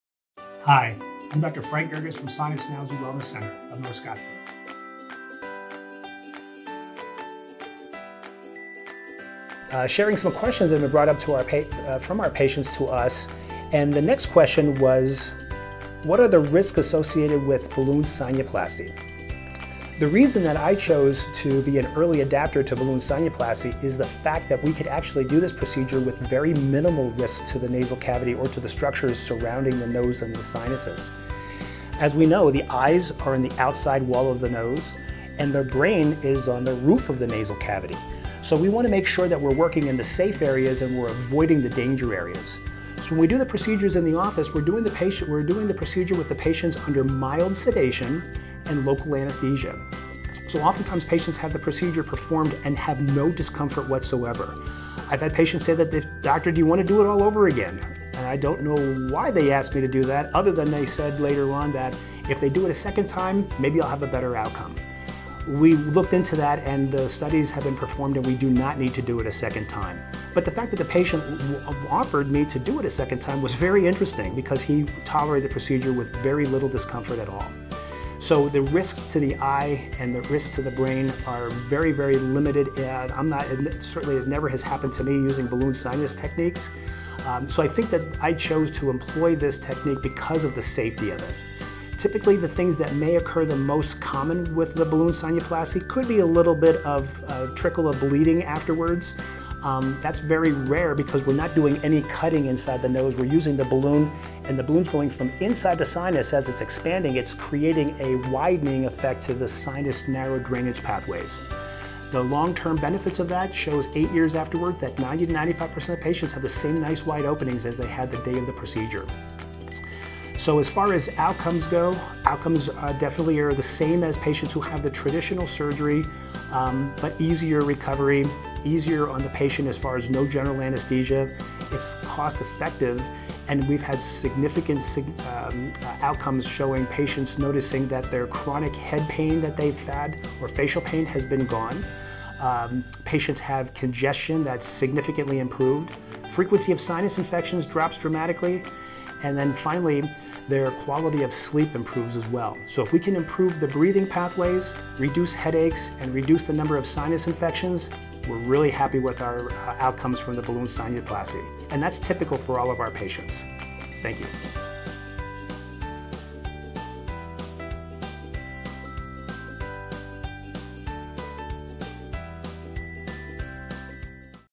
Are there risks associated with Balloon Sinuplasty?
Dr. Gergits was an early pioneer of the procedure because there is very MINIMAL risk to the nasal cavity or to the structure surrounding the nose and the sinuses.

The Balloon Sinuplasty procedure is performed in our office and our patient receives mild sedation and local anesthesia.

Oftentimes, the patient will experience no discomfort whatsoever.
The things that may occur may be a trickle of bleeding afterwards but that is very rare because we aren’t doing any cutting.

In 95% of Dr. Gergits’ patients, they have the same wide opening 8 years later after the procedure was first performed.

The outcome is the same for patients who have traditional sinus surgery but EASIER RECOVERY and easier on the patient because there is NO GENERAL ANESTHESIA, it’s COST EFFECTIVE.

Our patients can breathe better but also notice that their chronic head and facial pain goes away, congestion improves, and the frequency of sinus infections drops significantly.  Their quality of sleep improves as well.

The balloon sinuplasty improves the breathing pathways, reduces headaches and number of sinus infections.  This outcome is typical for all our patients!